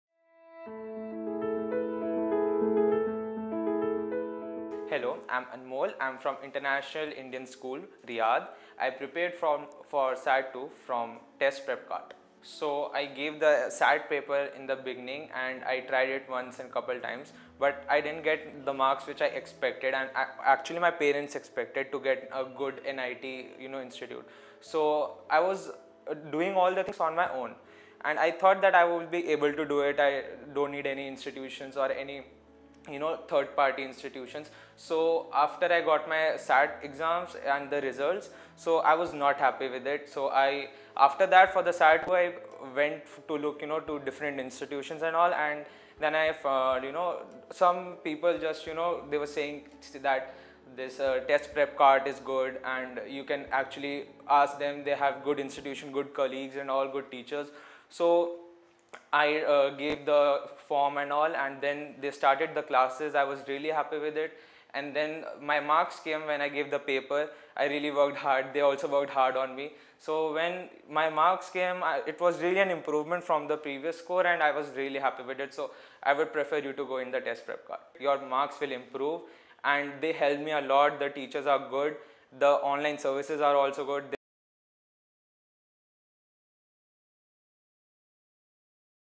Anmol SAT 2 Candidate (SAT Score 2350) - TestprepKart
Anmol who prepared for SAT 2 from Riyadh with us secured blazing 2350. In previous attempt he manage to get 1700 only.

Team TestprepKart is proudly presenting Anmol's story for other NRIs in Saudi Arabia for SAT / JEE / NEET preparation.